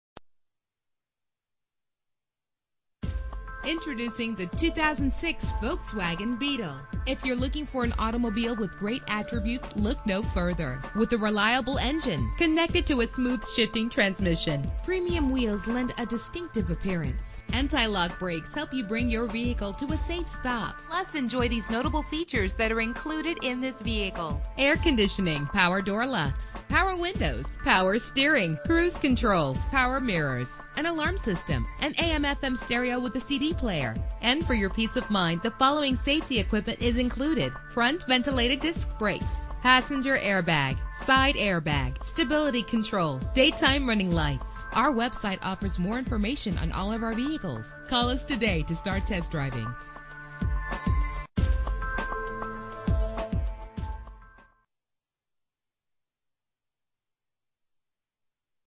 2006 Volkswagen Beetle - Longmont CO
Year: 2006
Make: Volkswagen
Model: Beetle
Trim: 2.5
Engine: 2.5 liter inline 5 cylinder 20 valve
Transmission:
Color: Blue
Mileage: 61075
Address:
1005 Ken Pratt Blvd